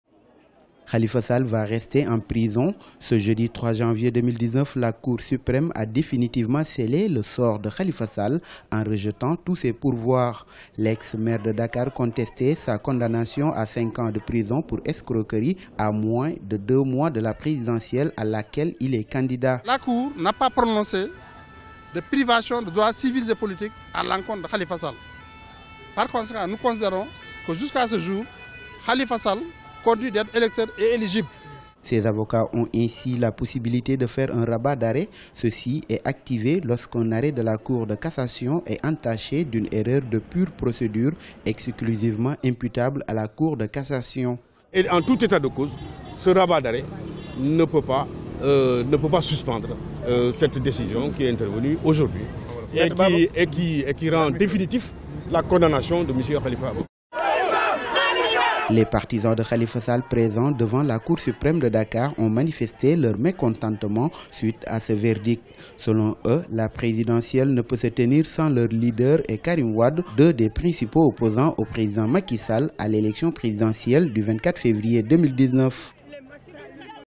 Sénégal, KHALIFA SALL DÉFINITIVEMENT CONDAMNÉ
Politique, La Cour Suprême rejette le recours de Khalifa Sall